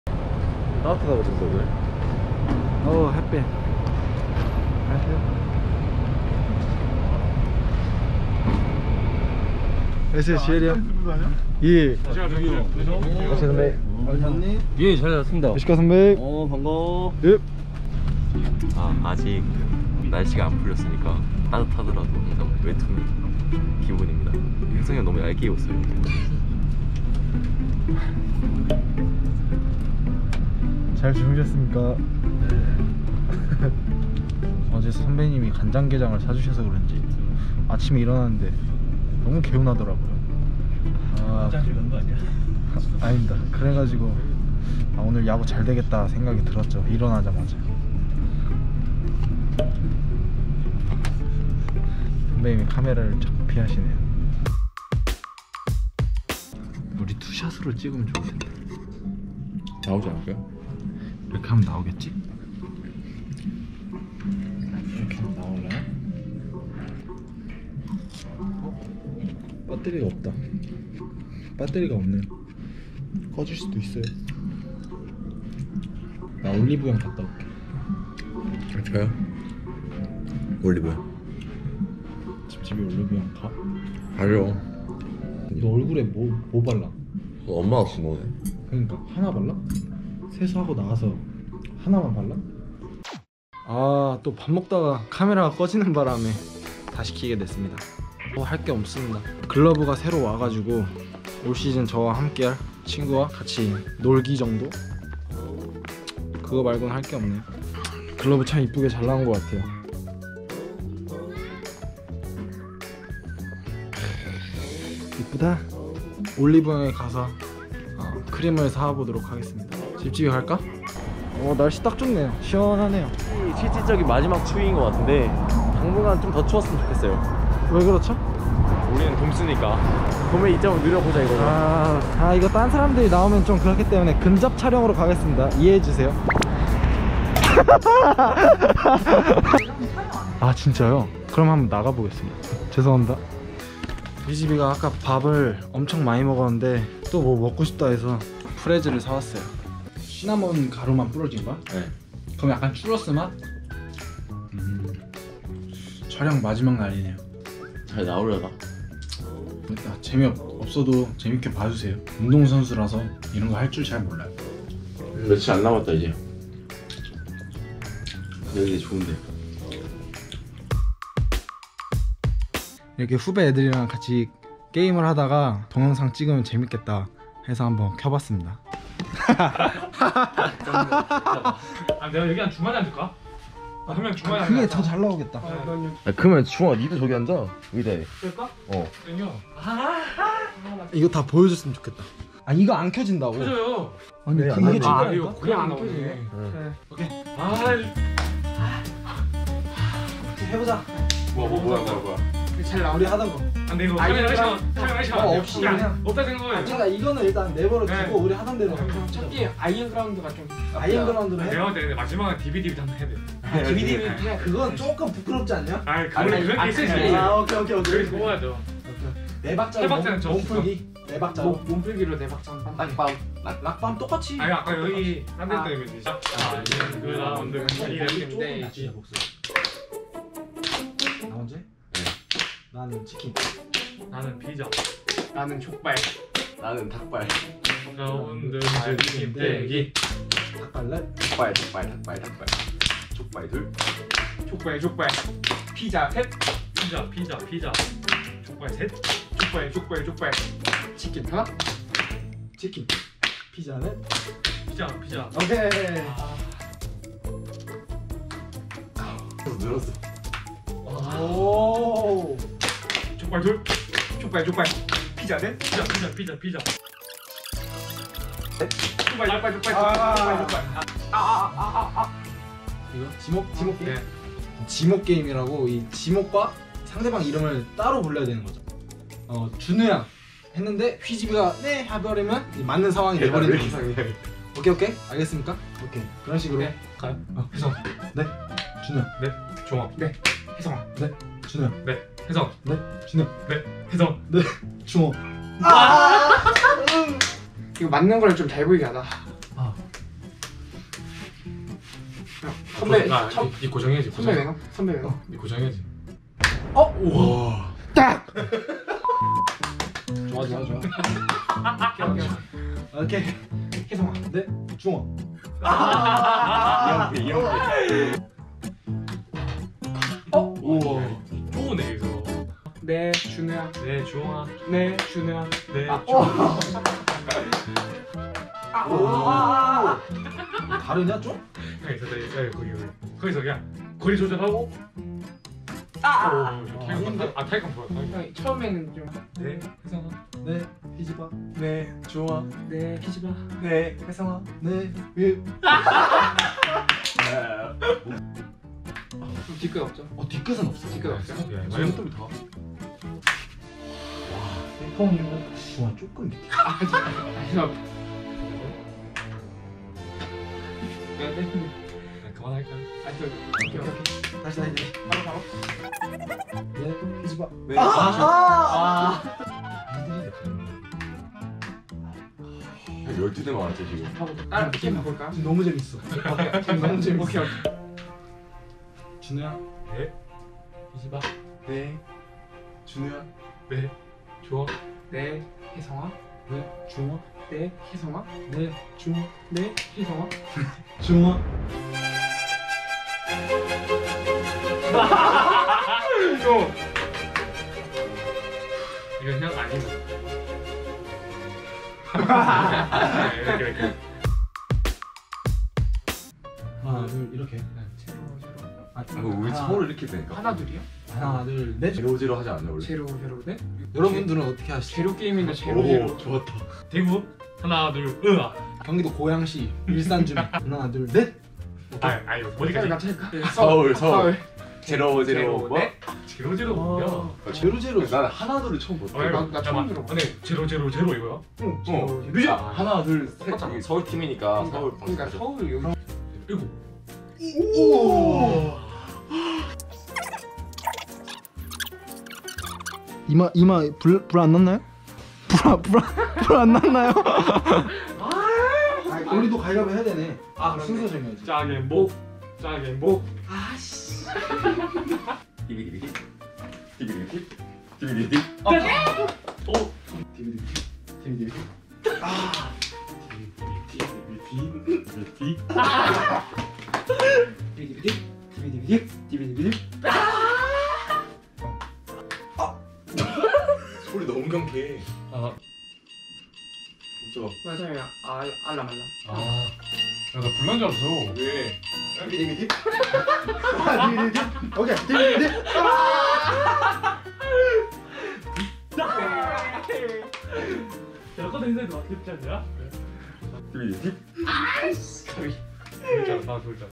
기분 좋게 후배들과 게임 한판 했습니다. 그런데 말입니다 | 히어로그 ep.11 김혜성 3편
♬너무 아픈 게임은 게임이 아니었음을...🤕